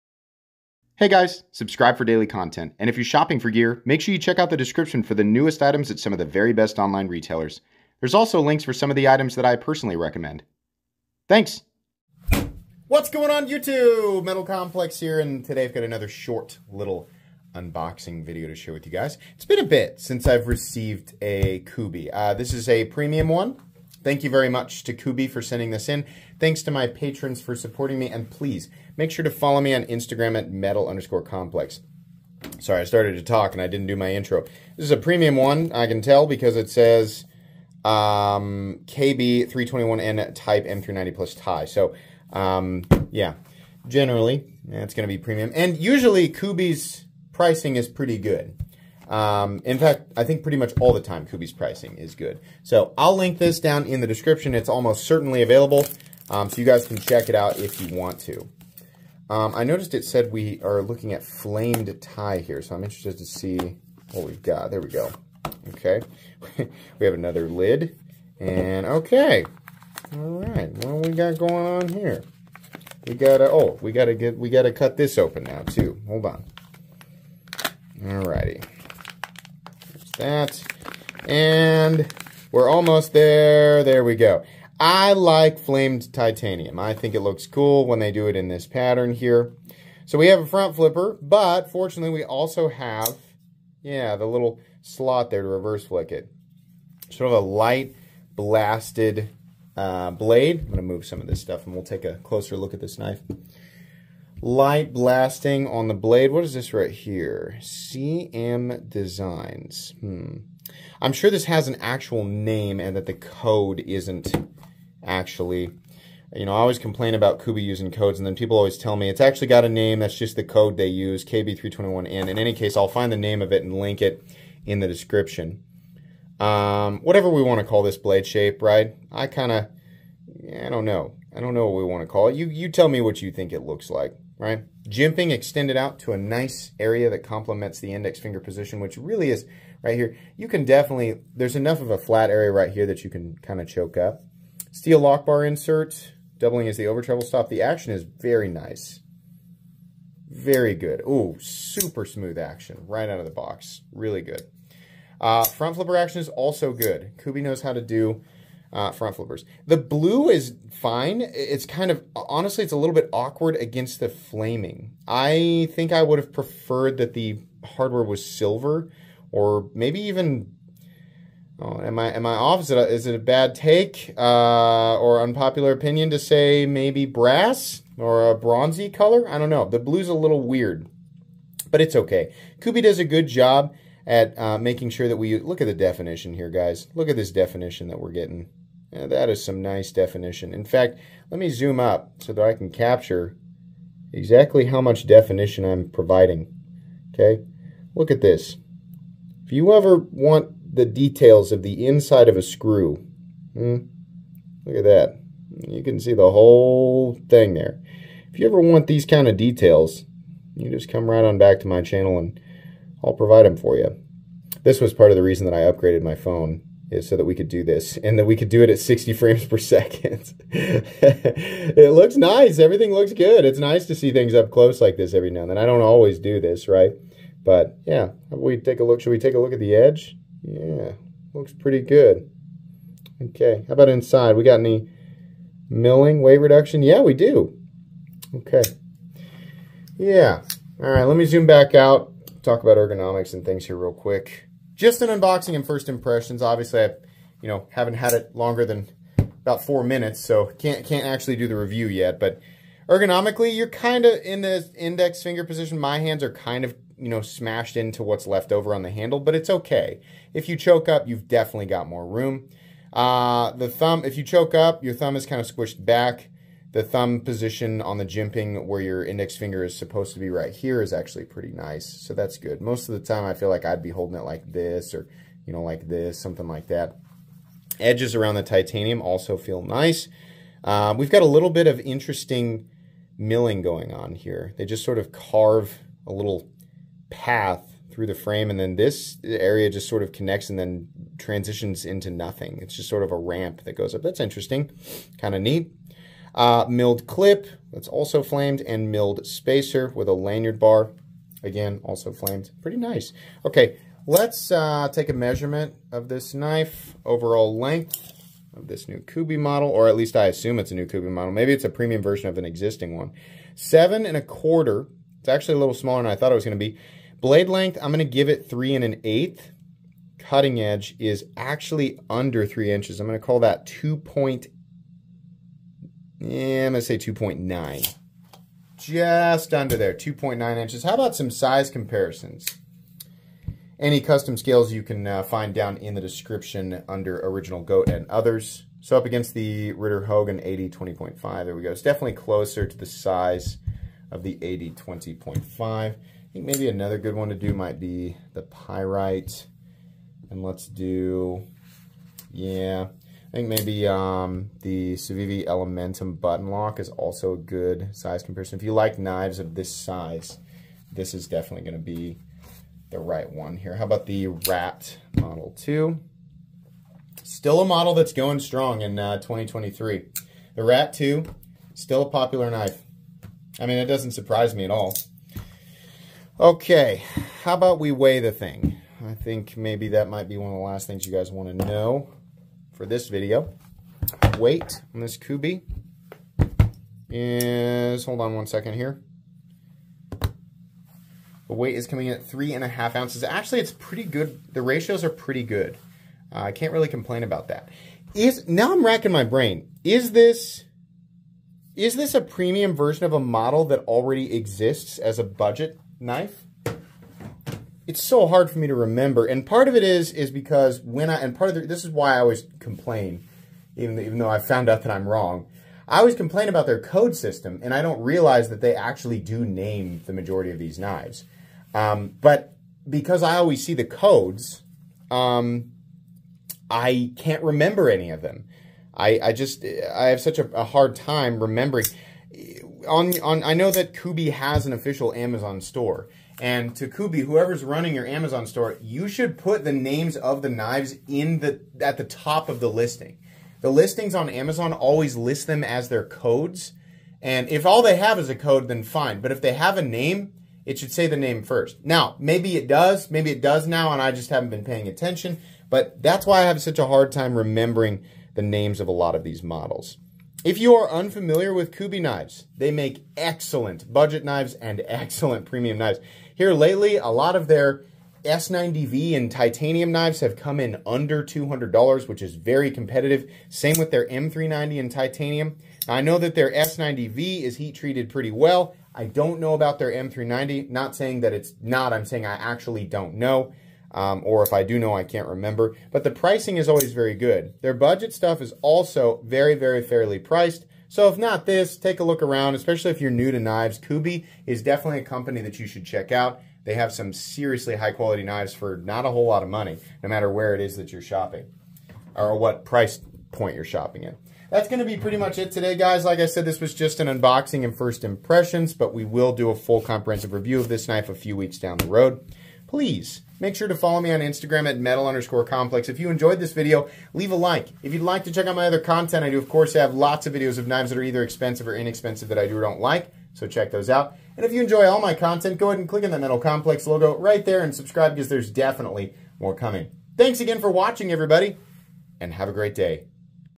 I KNEW This Knife Looked Familiar... They UPGRADED It!
Unboxing the premium Kubey Royal.

Kubey Royal

Polish Custom Knives New Arrivals!

Check out Tools For Gents!

Shop Flytanium! Custom parts for your favorite knives!